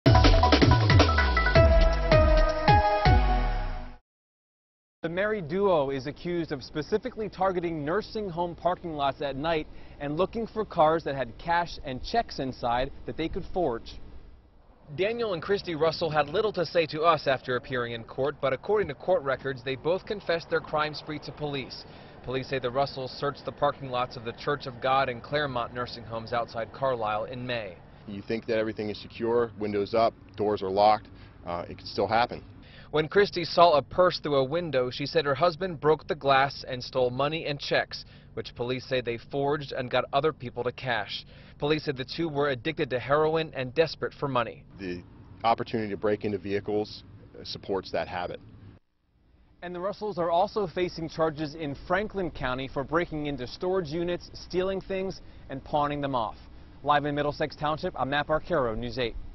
Police: Couple admits to car break-in spree; duo targeted nursing home lots
Police say money for heroin led to numerous car break-ins in several counties. A married duo, Daniel and Christy Russell, is accused of targeting nursing home parking lots at night to look for cash and checks that they could forge.